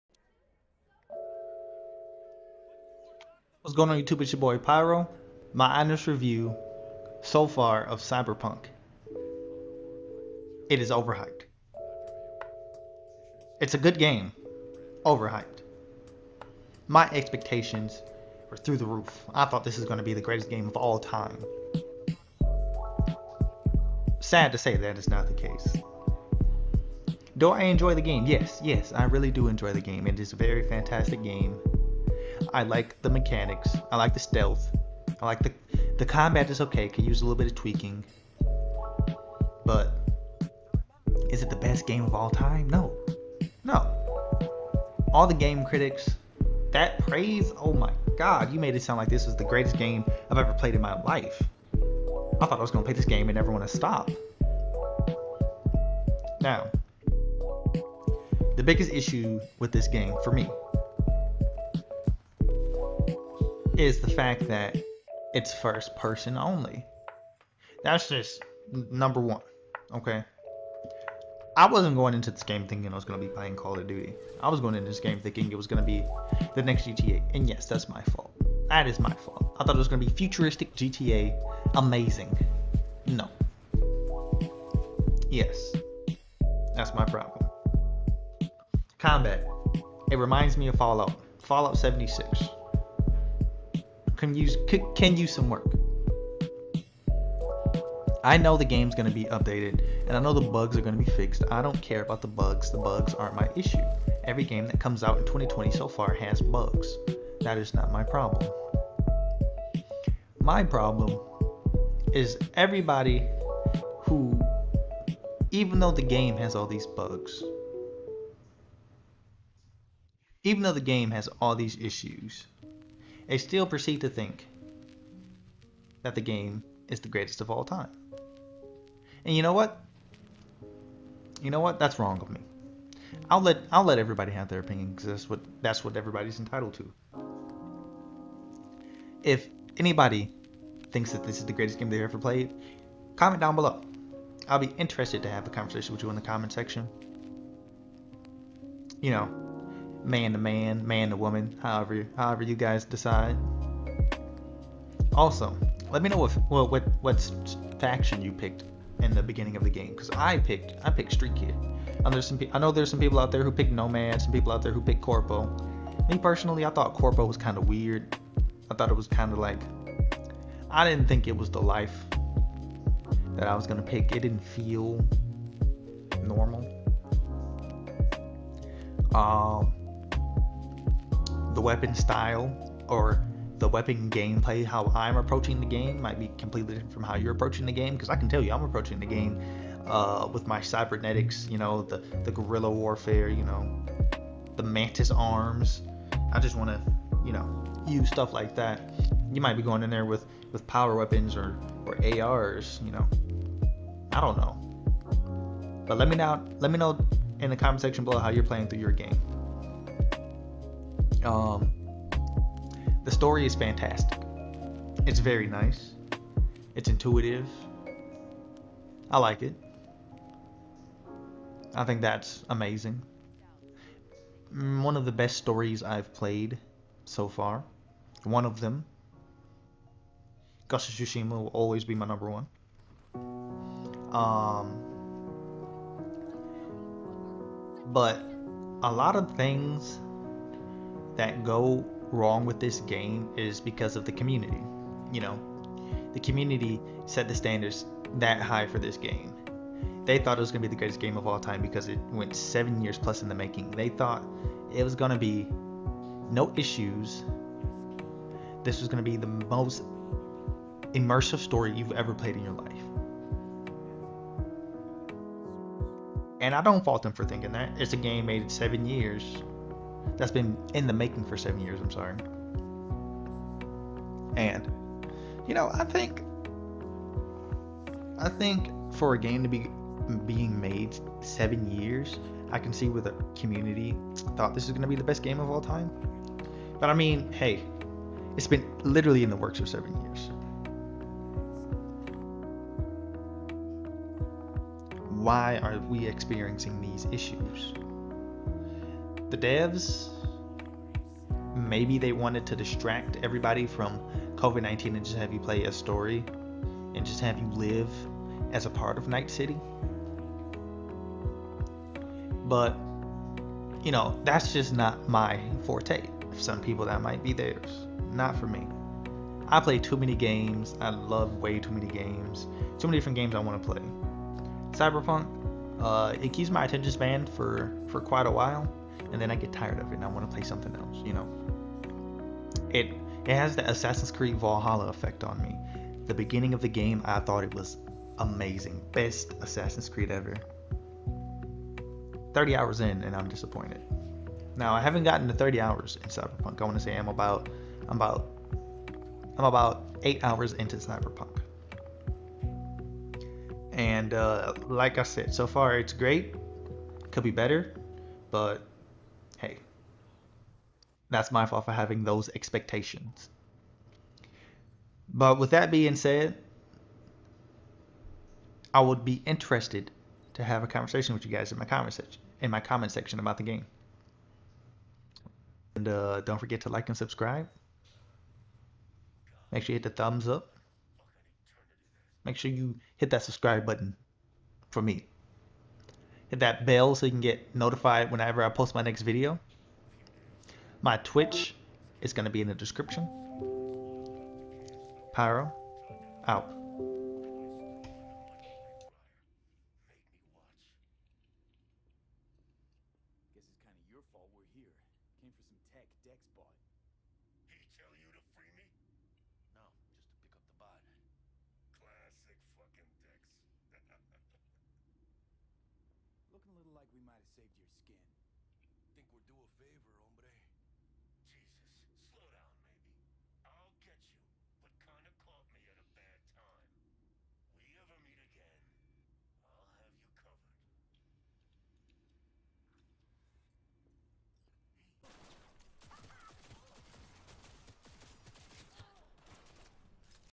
Honest Review of Cyberpunk 2077
My First Honest Review of Cyberpunk 2077 go easy on me guys. I hope you all enjoy the video.

How To free Brick: 5:55